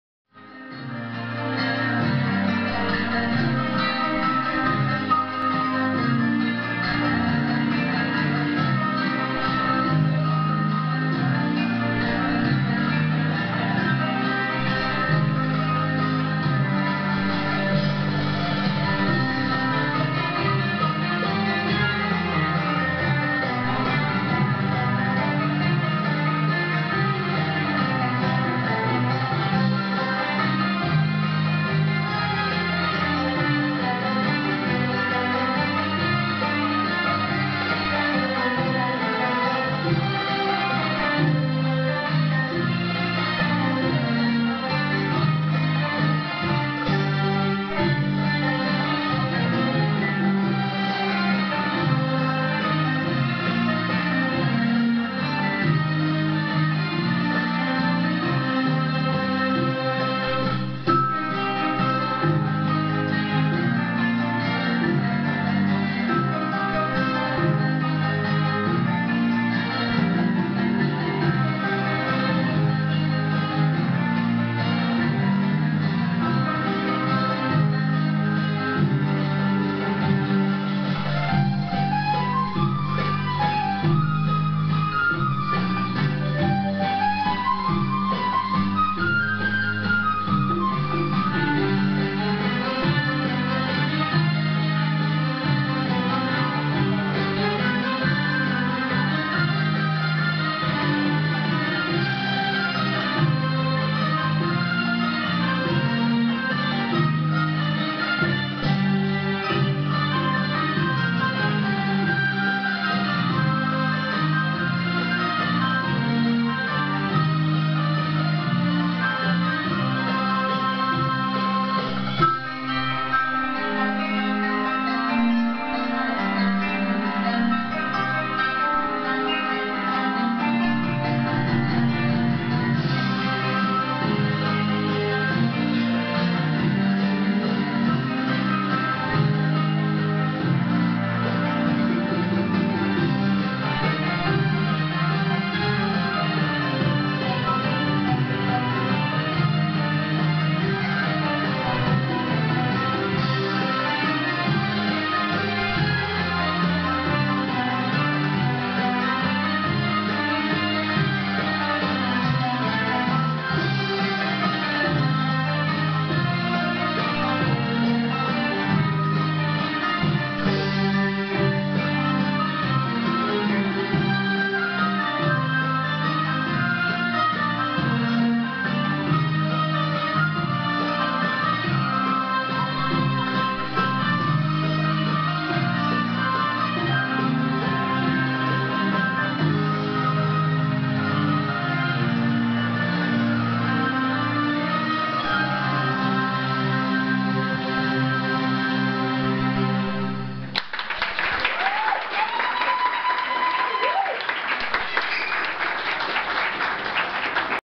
DANÇA COM VÉU
Apresentação com minhas alunas.
Bailarinas: Eliane Magalhães e Odile Ugarte. Bjs pra vcs, minhas queridas! Parabéns pela apresentação!